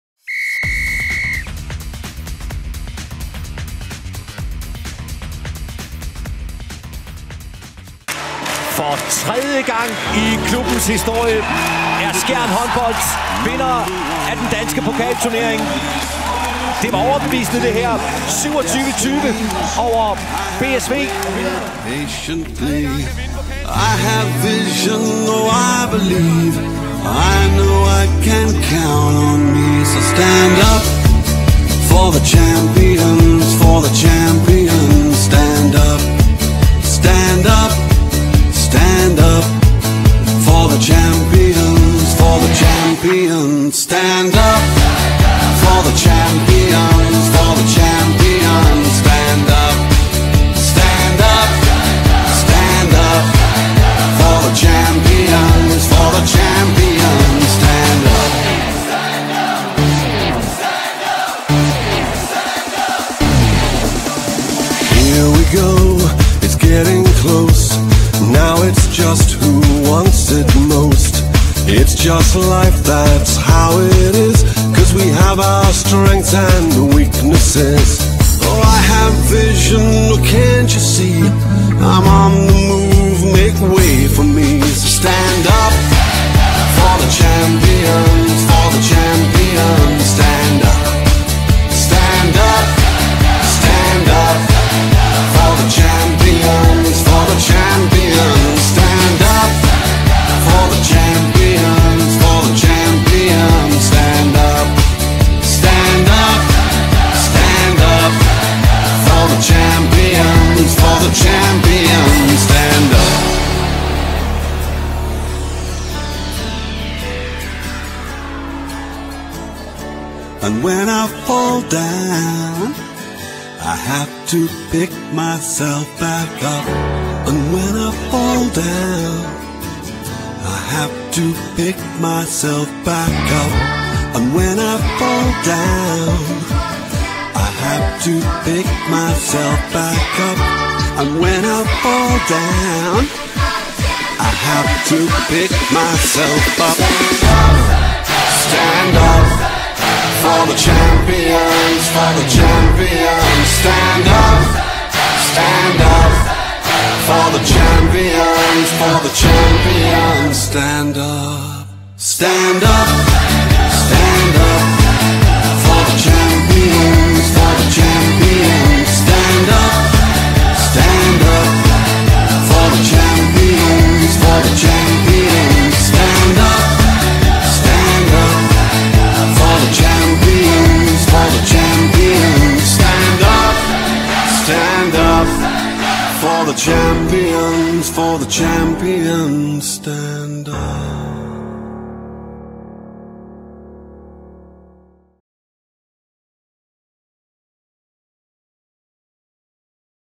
Pokalmestre 2015/16
Skjern Håndbold som Pokalmestre 2015/16 - finalen blev vundet over BSV og blev spillet i Gigantium, Aalborg.